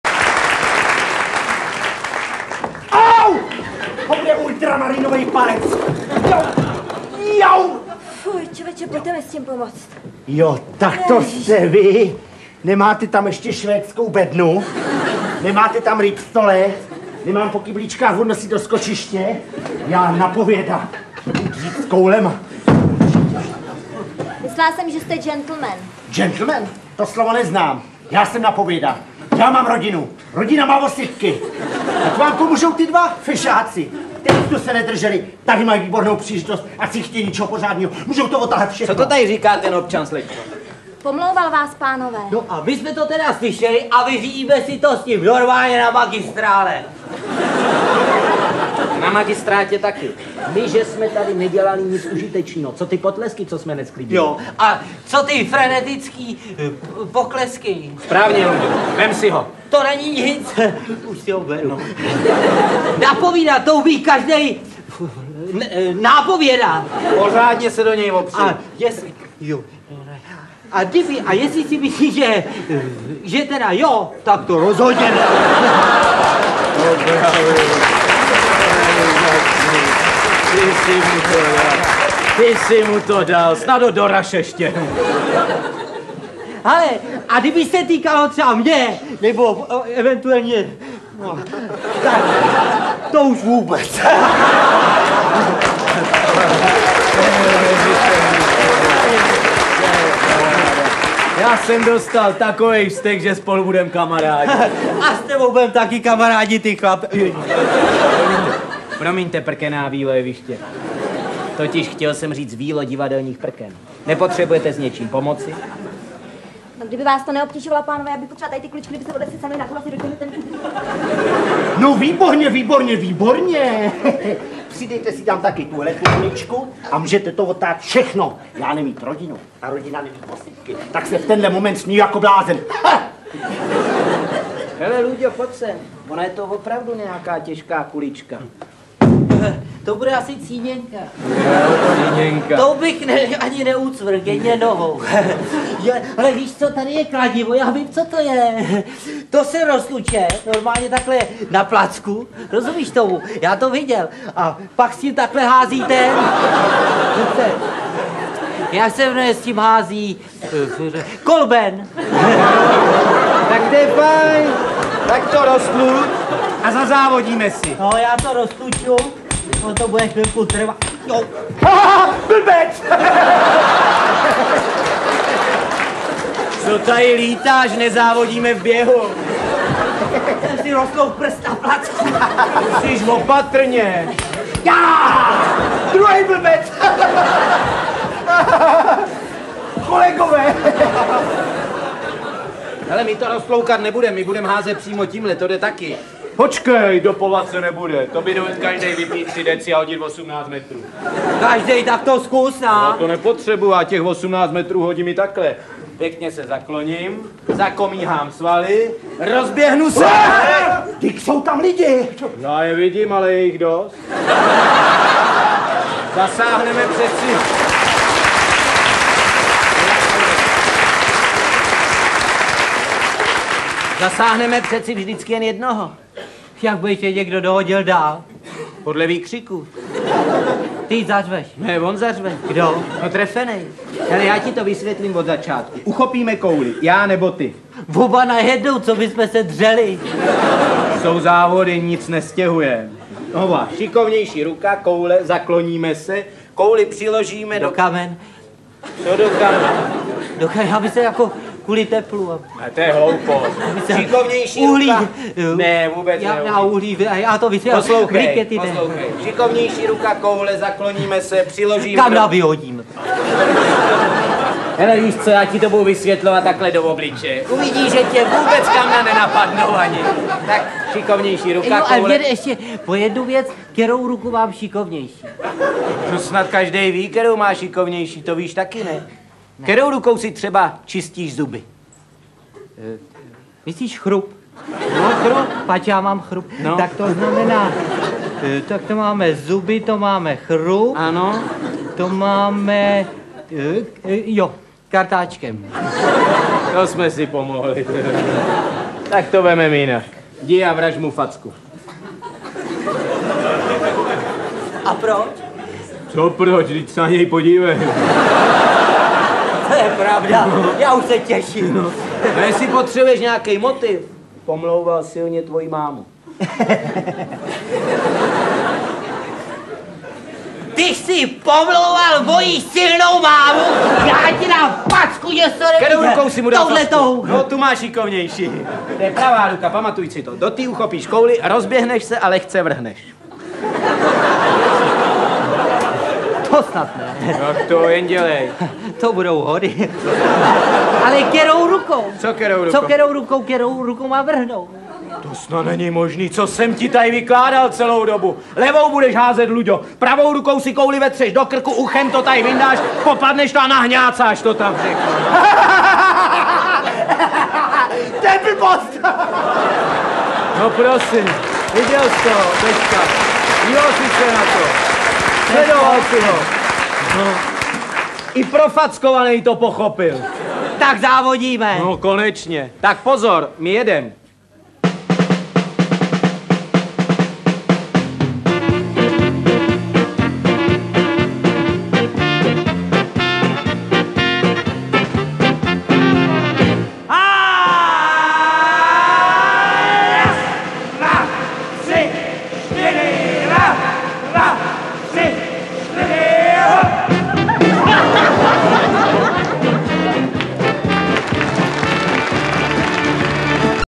Sobota, Šimek, Nárožný - koule
Jedna z nejlepších scének téhle trojice, která se boužel neobjevila ani na DVD s jejich scénkami.